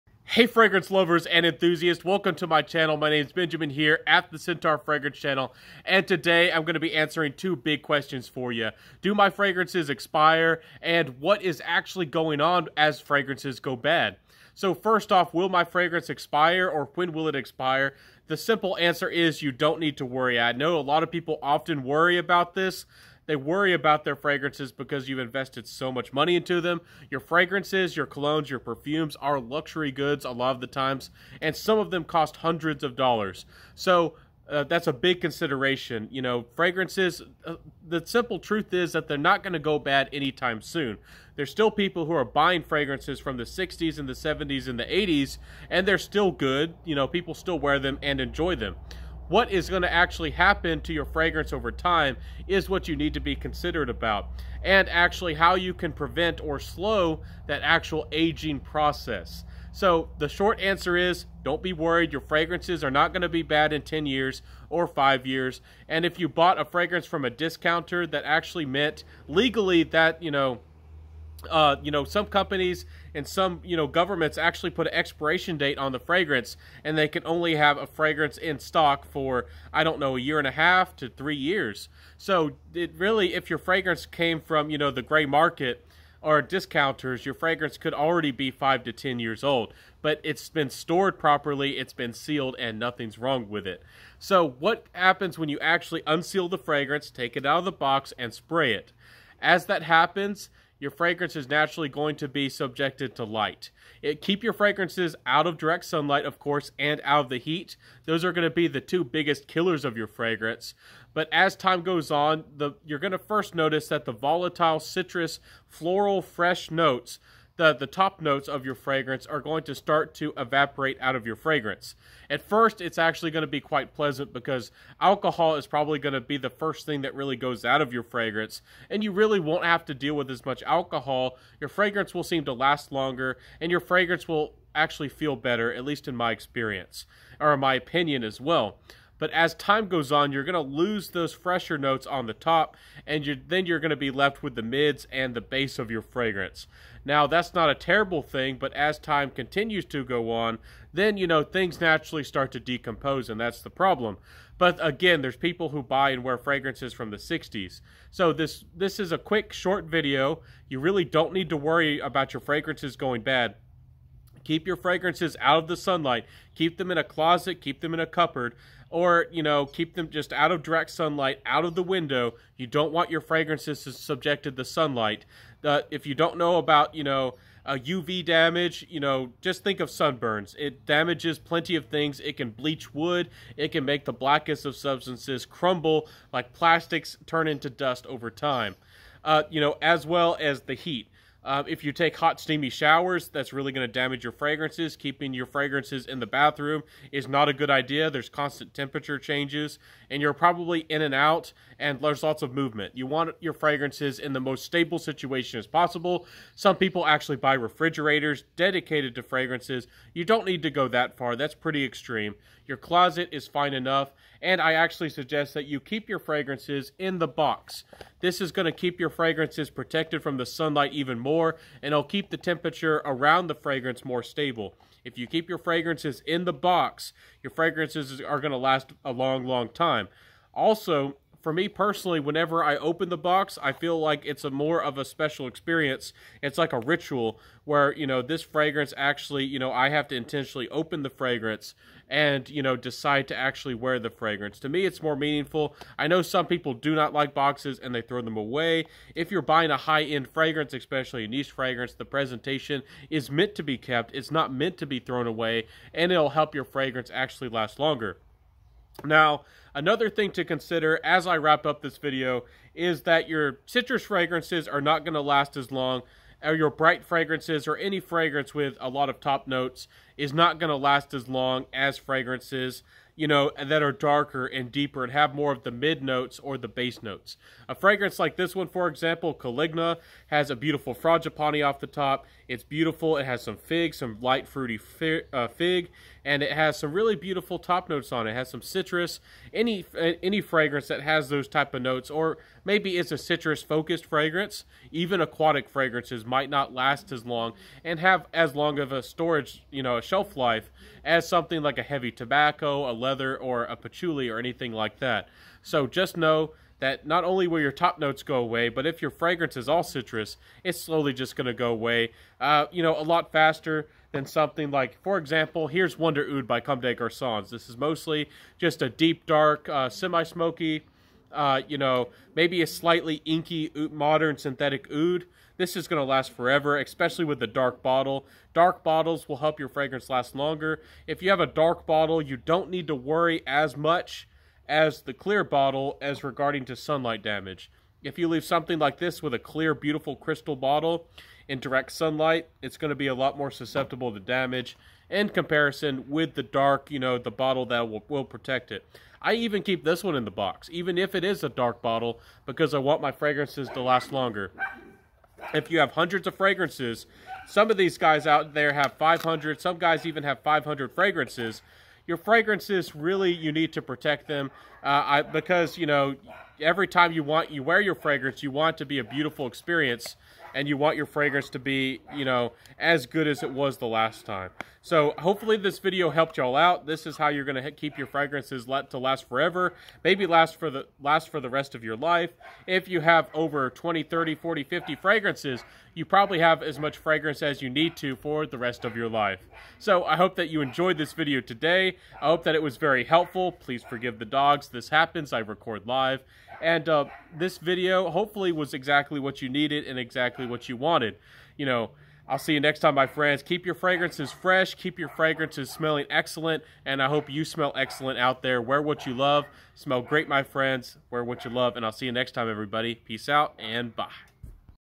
Will my Fragrance go Bad ? When will my Fragrance Expire ? - Big Fragrance Chat !
I know this problem can get stressful , especially when you have 100 or more fragrances. You have so many fragrances and you want them to last for a long time ! I am here to help !